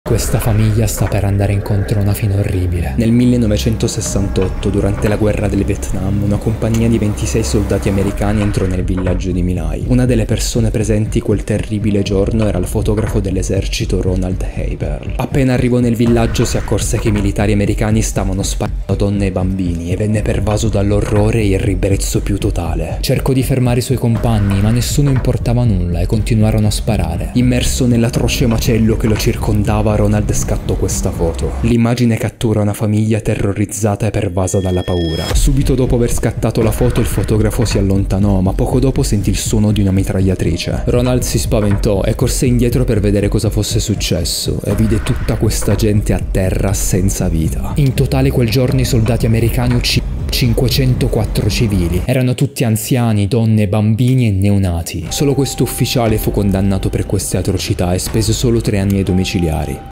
Il Terribile Caso Di My Lai
La storia del villaggio di My Lai e del fotografo Ronald Haeberle.